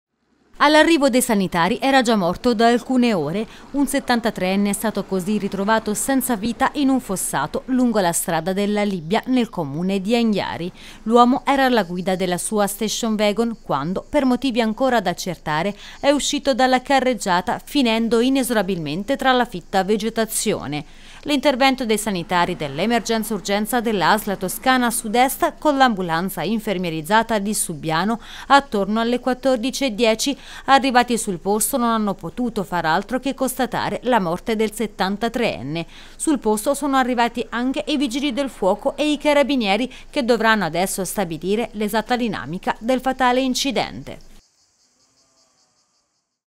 Anghiari, trovato senza vita all’interno della propria auto in un fossato
All'arrivo dei sanitari era già morto da alcune ore.
Un 73enne è stato ritrovato senza vita, in un fossato, lungo la strada della Libbia, nel comune di Anghiari.
L'uomo era alla guida della sua station wagon, quando per motivi ancora da accertare, è uscito dalla carreggiata, finendo inesorabilmente tra la fitta vegetazione.
Alle 14,20 l'intervento dei sanitari del 118. Sul posto anche vigili del fuoco e carabinieri.